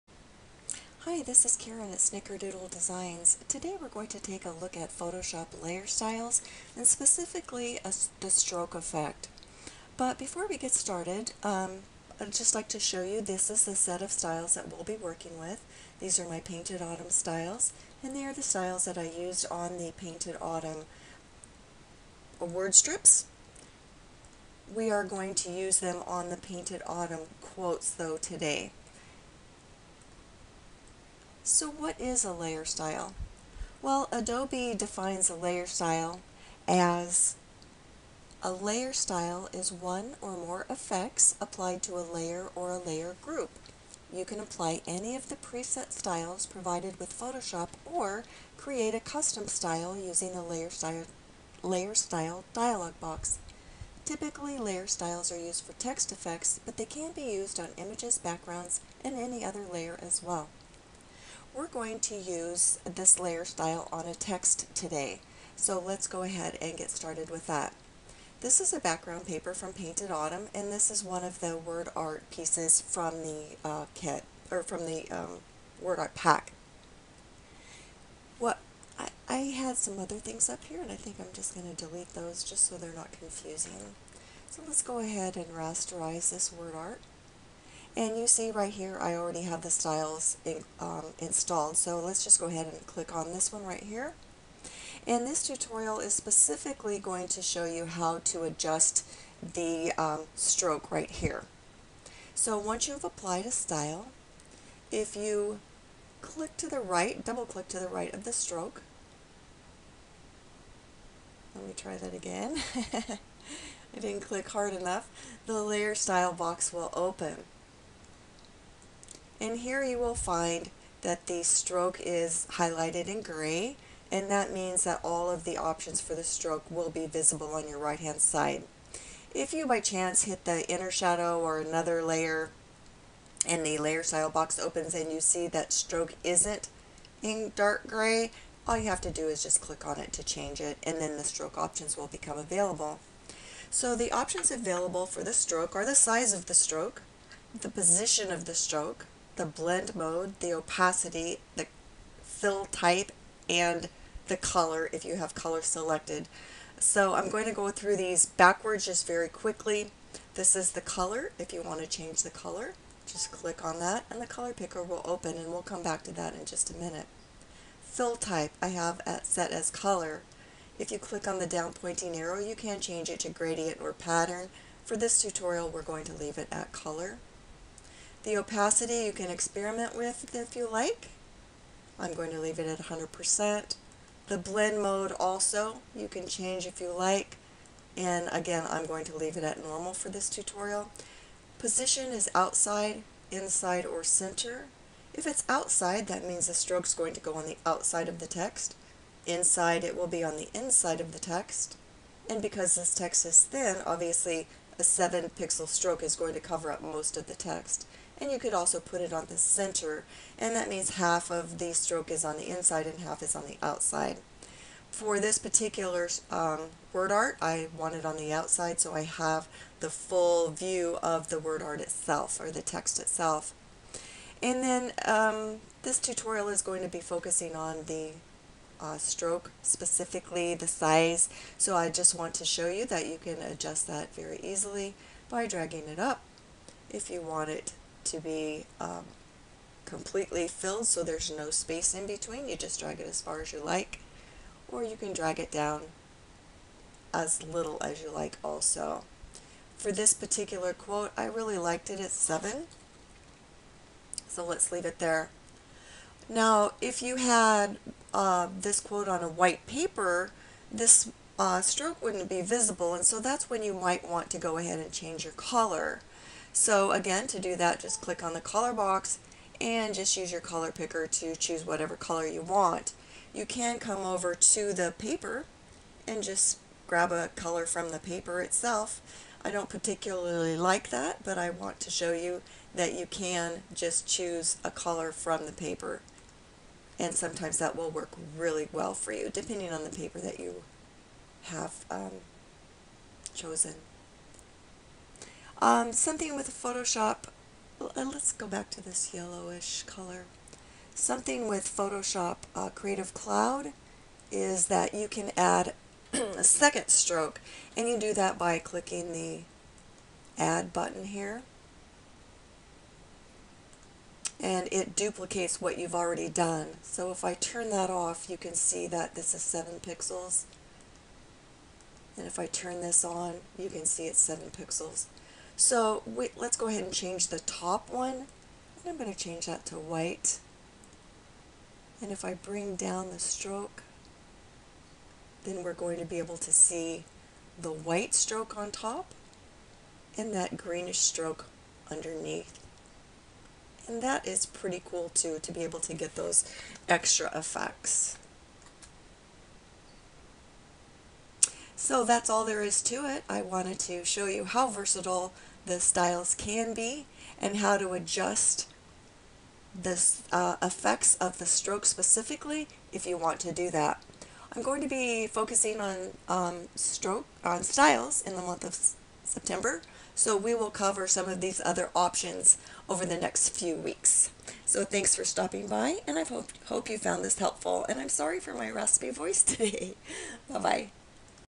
How to Adjust the Stroke Effect in a Photoshop Layer Style
Find Painted Autumn products at
How to Customize Word Strips and Word Art, using my Painted Autumn Collection.